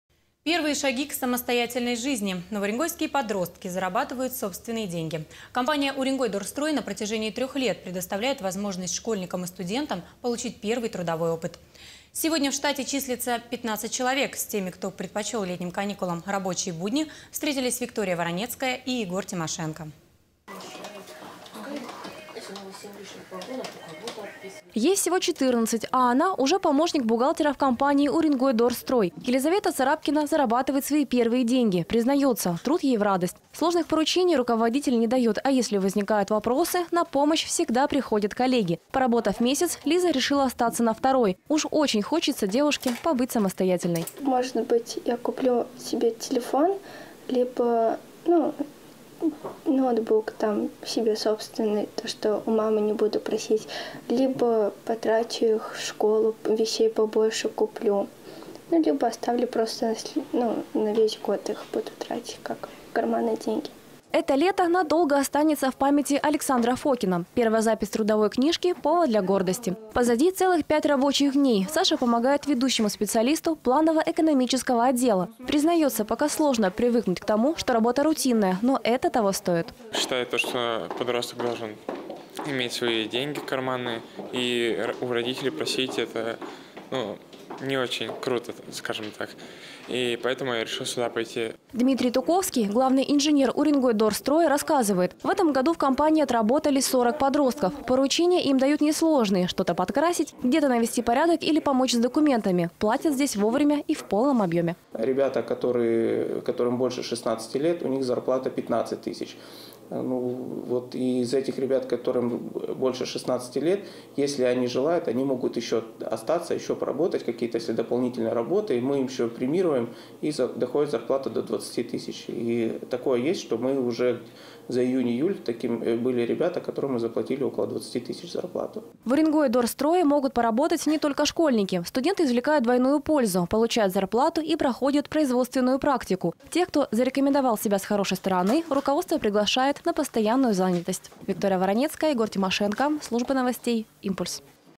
Новоуренгойские подростки зарабатывают первые деньги на городских предприятиях
Первые шаги к самостоятельной жизни. Новоуренгойские подростки зарабатывают собственные деньги. Компания «Уренгойдорстрой» на протяжении трёх лет предоставляет возможность школьникам и студентам получить первый трудовой опыт. Сегодня в штате числятся 15 человек. С теми, кто предпочел летним каникулам рабочие будни, встретились Виктория Воронецкая и Егор Тимошенко.